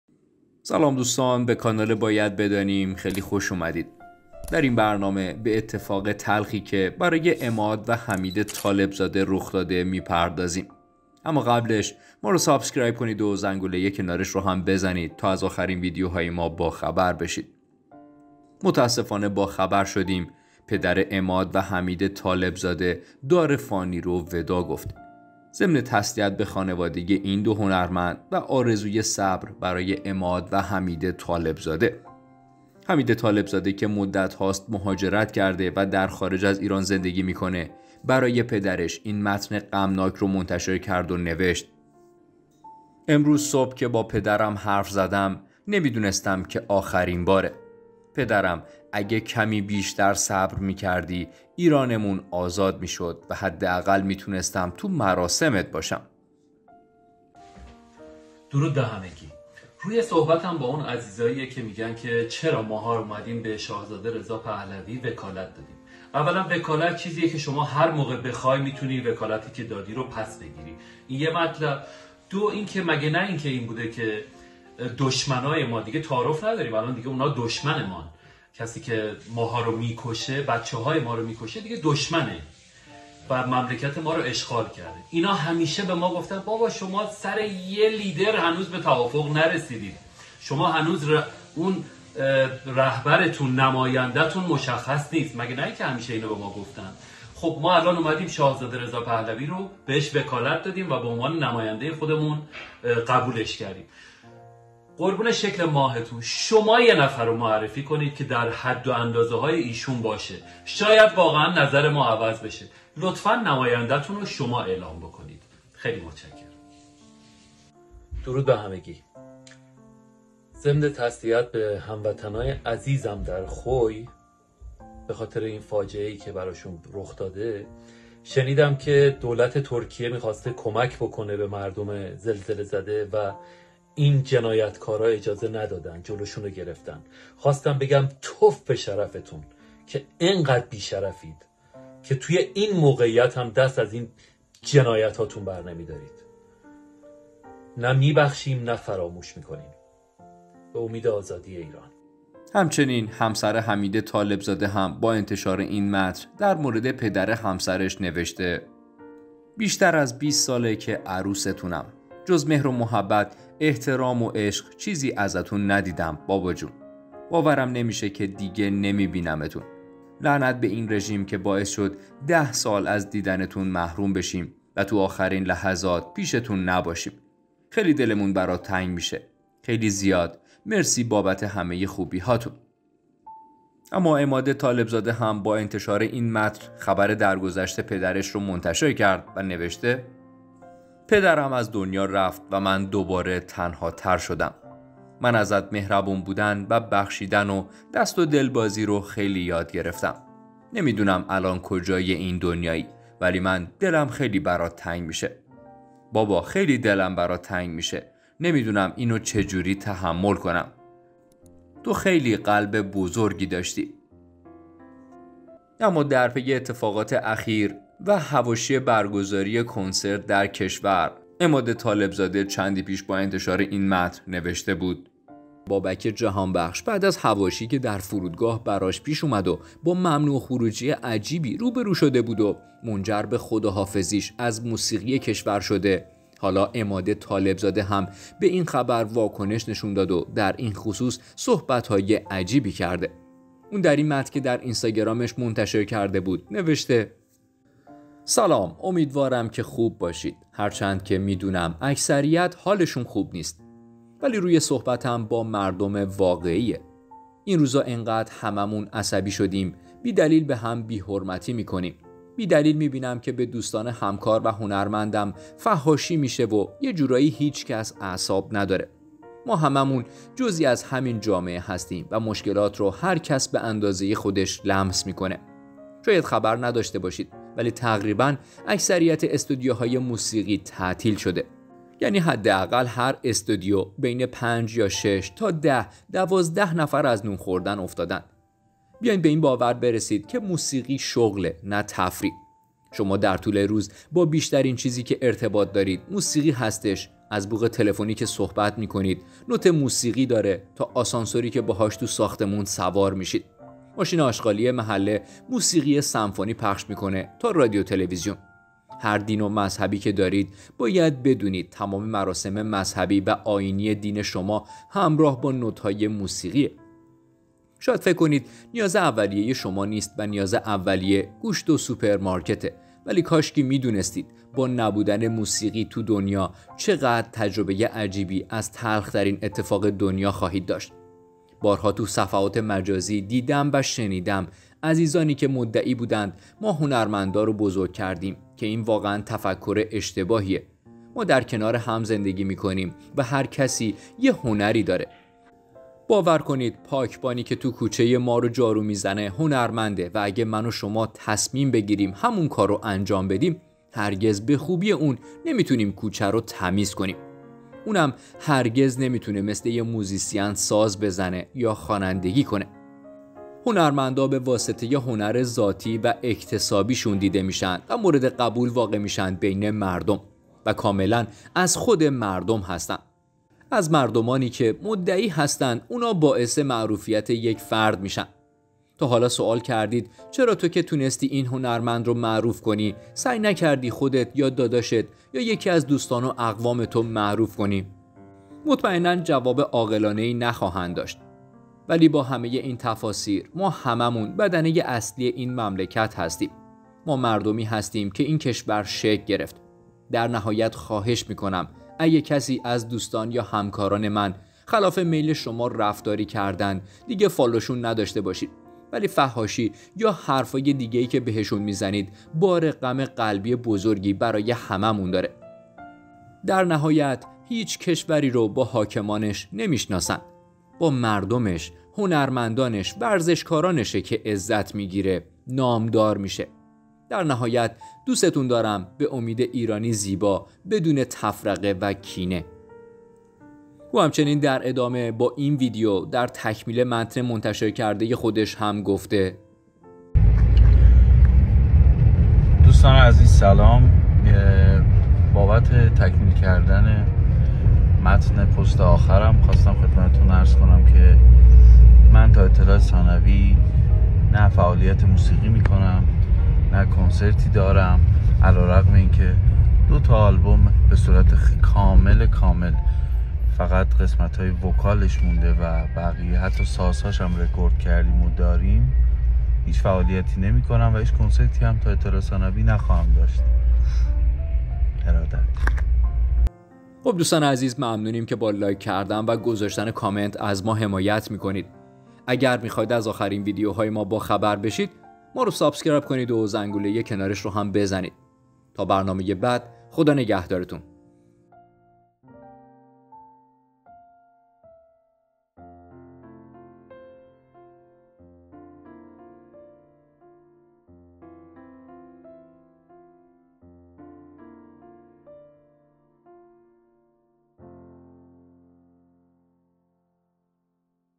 فوری: اتفاقی تلخ برای حمید طالب زاده!
متاسفانه با خبر شدیم پدر عماد و حمید طالب زاده دار فانی رو وداع گفت .ضمن تسلیت به خانواده این دو هنرمدن و ارازوی صبر برای عماد و حمید طالب زاده . احمید طالب زاده که مدتهاست مهاجرت کرده و در خارج از ایزان زندگی میکنه  برای پدرش این متن غمناک رو  منتشر کرده  ونوشته
👇از اینجا عضو کانال ما شوید و سابسکرایب کنید👇